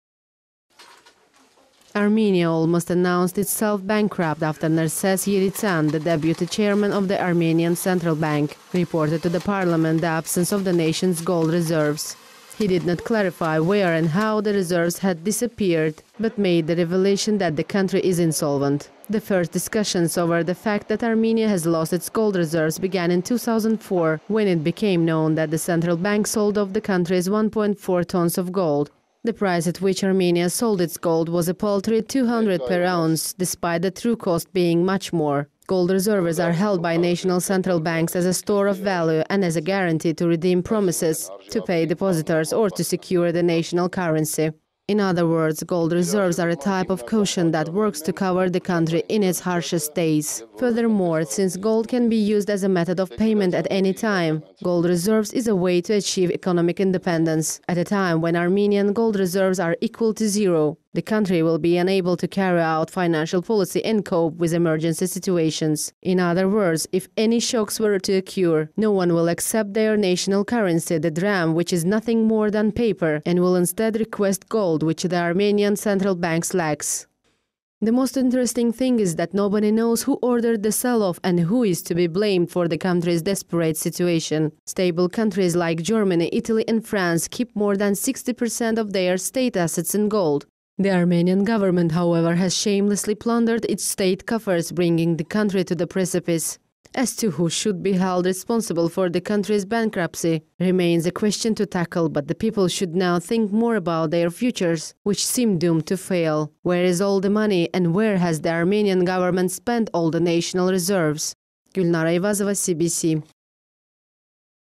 Armenia almost announced itself bankrupt
Armenia almost announced itself bankrupt after Nerses Yeritsyan, the Deputy Chairman of the Armenian Central Bank, reported to the parliament the absence of the nation’s gold reserves.

He did not clarify where and how the reserves had disappeared, but made the revelation that the country is insolvent.

The first discussions over the fact that Armenia has lost its gold reserves began in 2004, when it became known that the Central Bank sold off the country’s 1.4 tons of gold. The price at which Armenia sold its gold was a paltry $200 per ounce, despite the true cost being much more.